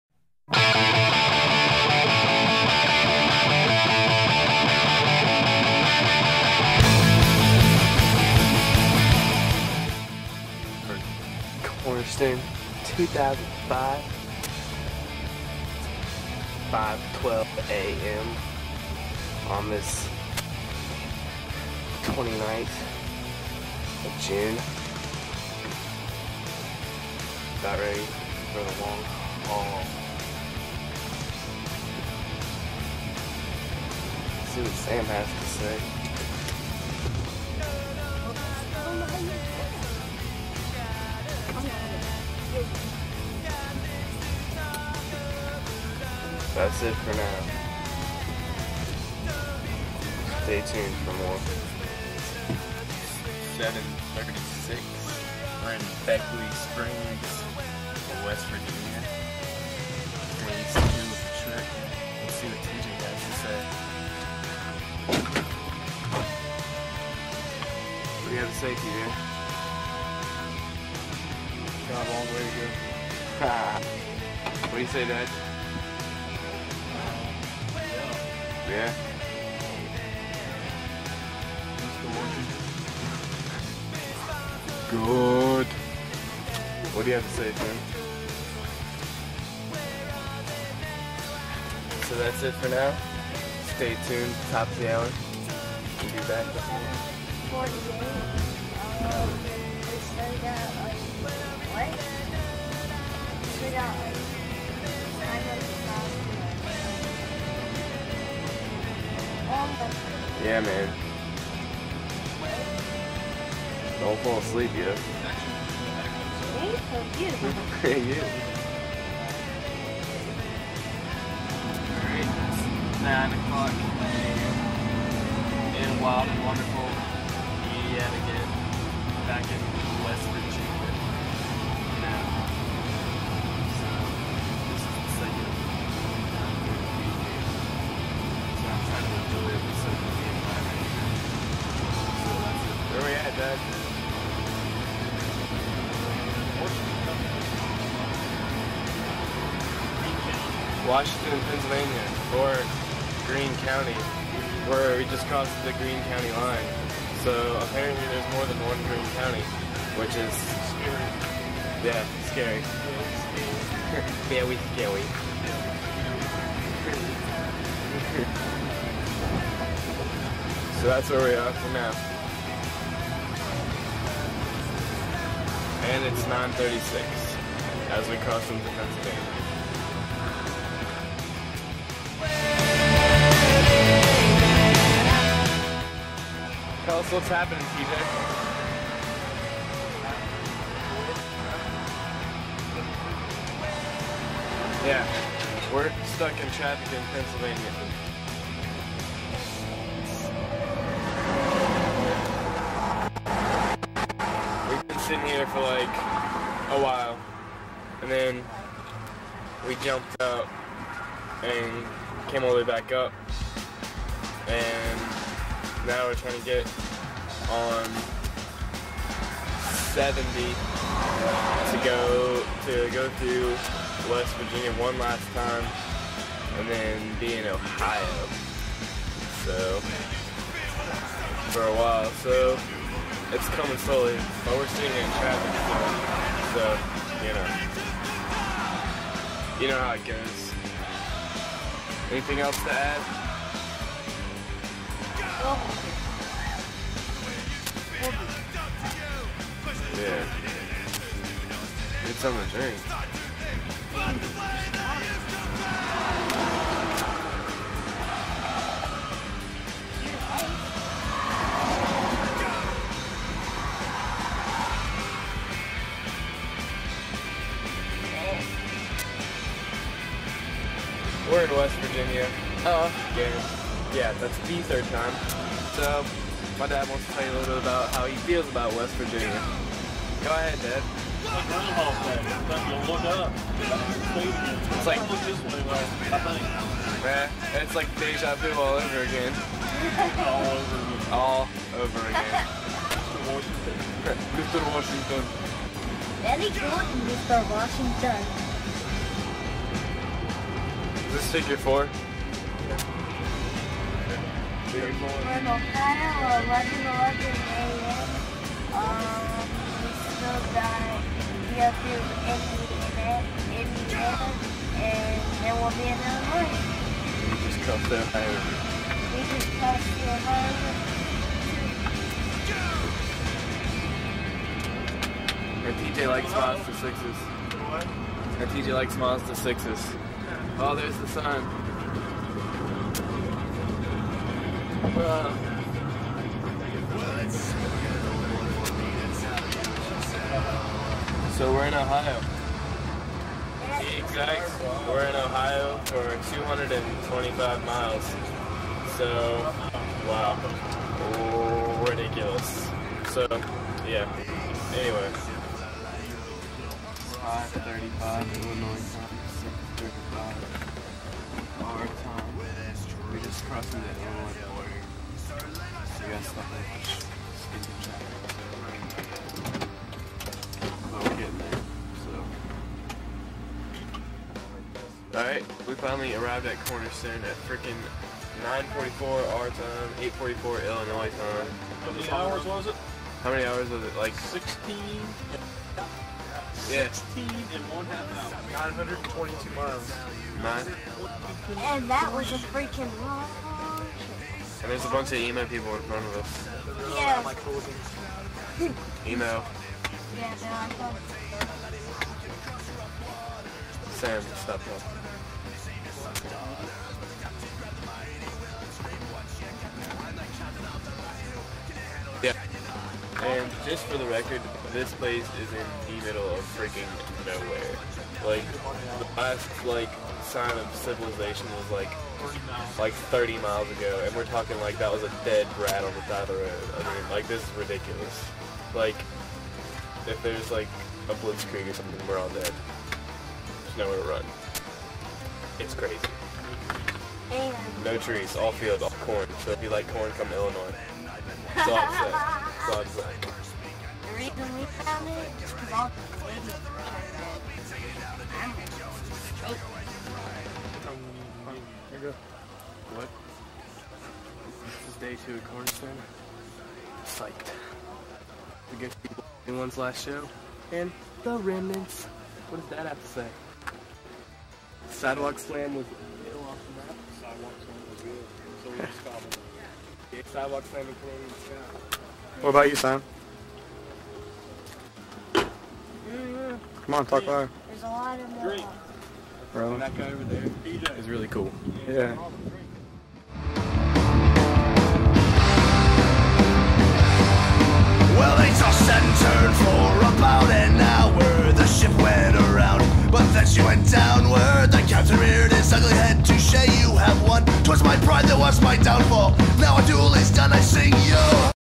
ROAD TRIP TO CORNERSTONE 2005
In 2005 Robert, TJ, Sam and myself drove from VA to Cornerstone in IL - this is some random video taken during that arduous journey